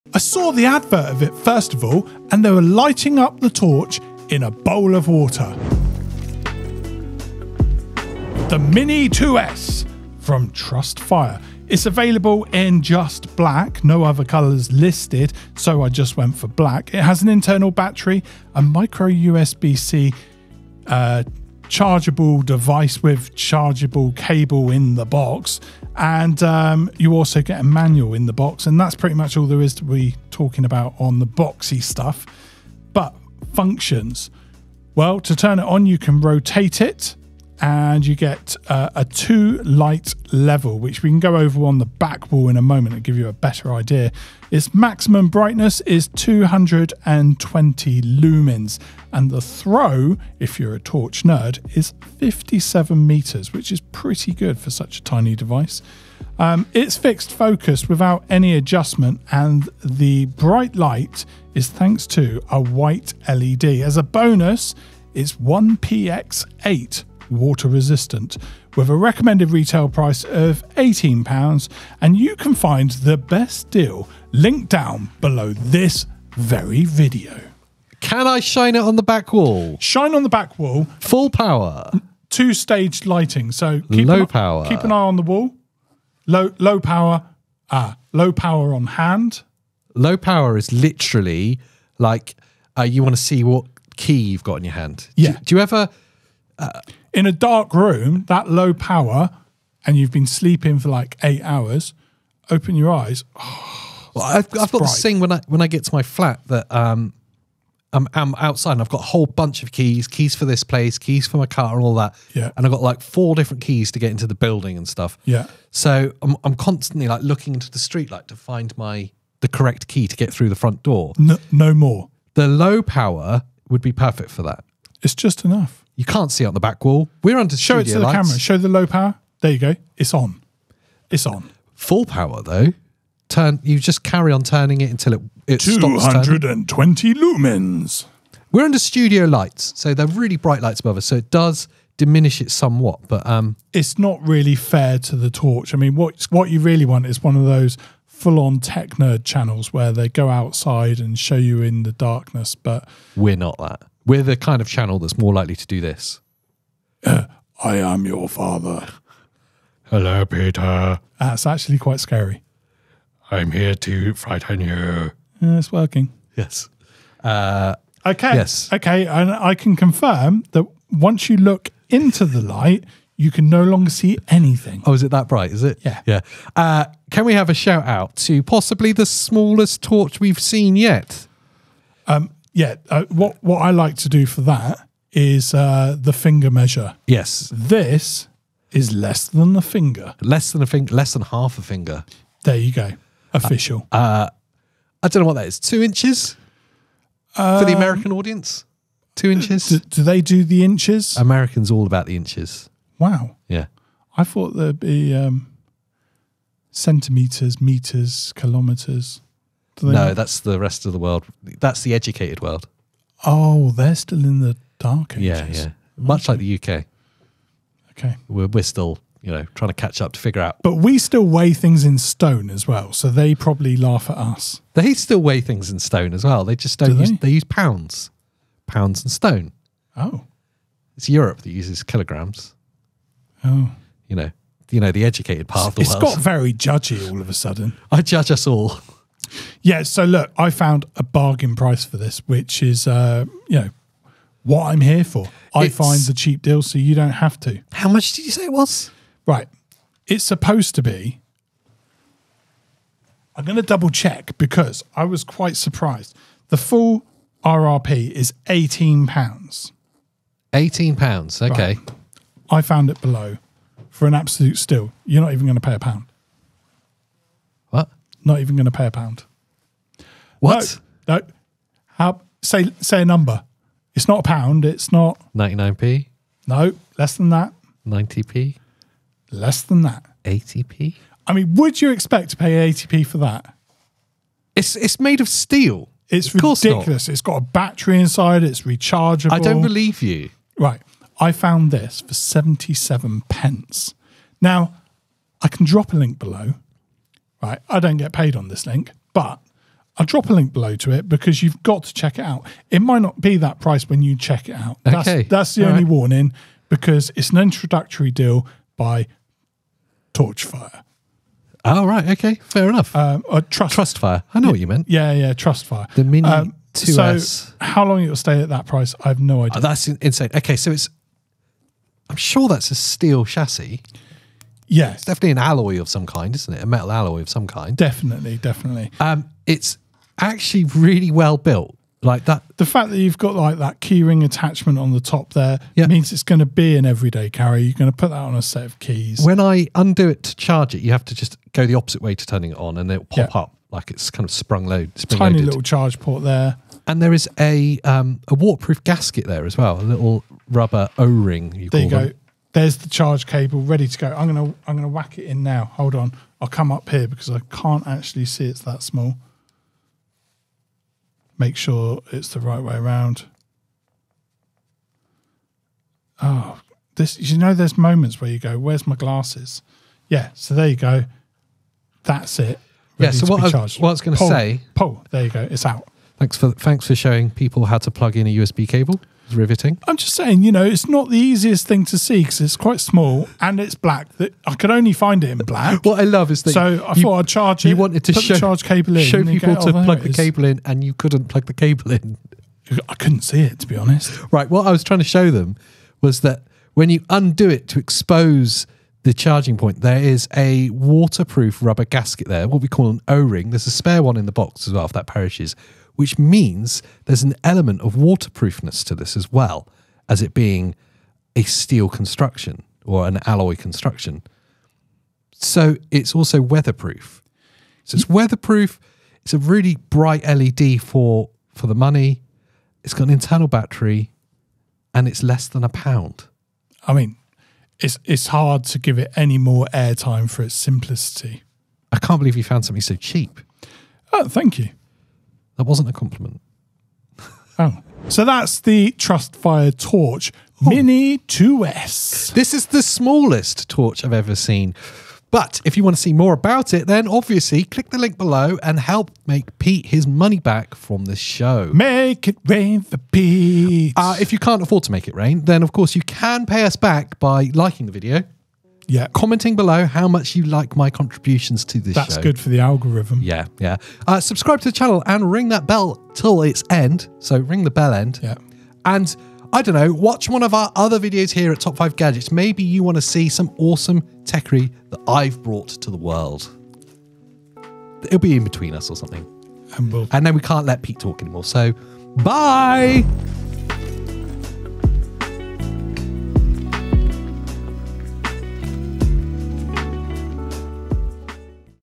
Smallest Ever Flashlight? Trustfire Mini2S
Trustfire Mini2S might be the Smallest Ever EDC Flashlight we have seen. We take a closer look at this mini everyday carry torch in our TrustFire Mini 2S Review.

🛒 Trustfire Mini2S Mini Torch
🏷 Amazon's Best Deal Today (Updated Daily)

Gear We Use In Our Videos
Podcast Mics
Røde mixing desk
Mic Stands